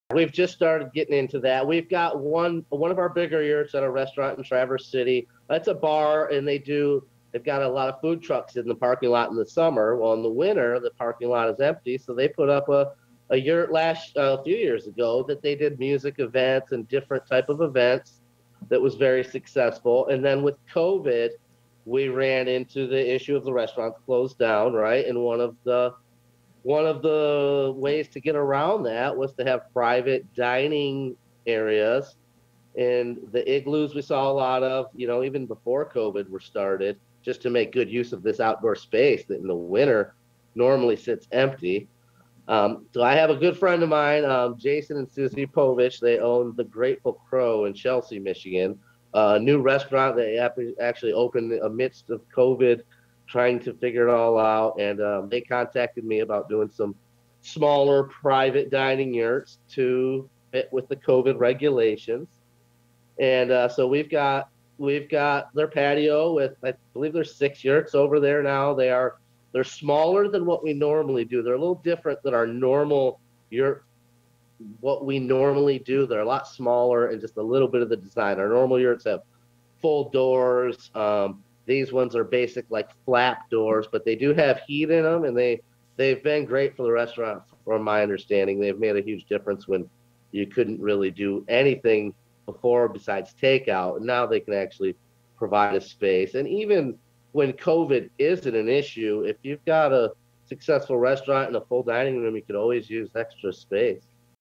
Great Lakes Yurt Co. Owner Says Yurts Utilized by Some Restaurants for Outdoor Dining
Great Lakes Yurt Co. Owner, Steve Caro, says that his company has been contacted by multiple restaurants about providing them with yurts to use as outdoor structures in order to provide additional outdoor dining as indoor in-person dining continues to be partially-restricted under current health orders.